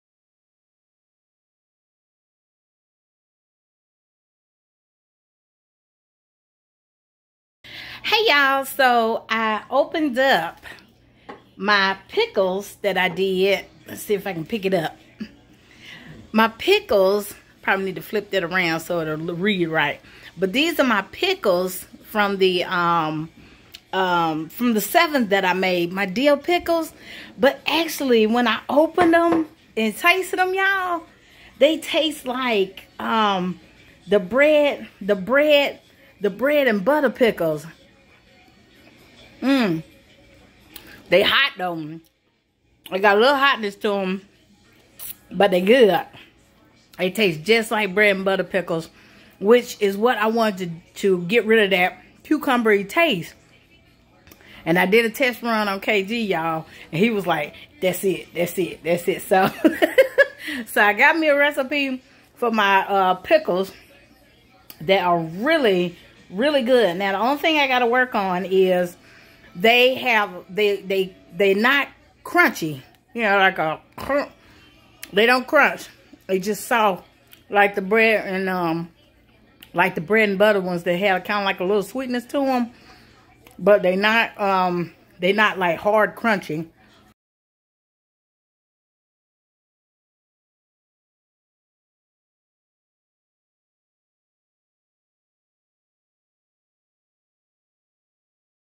first canned pickle tastetest - catshobbycorner
So I canned my harvested Cucumbers approximately 2 weeks ago. I decided to open them for a taste test today and I was shocked and pleased by the results! Loved the burst of sweetness & pop of hotness. Delicious! first canned pickle tastetest
Recipe courtesy of Youtuber Richey-Huskey Farm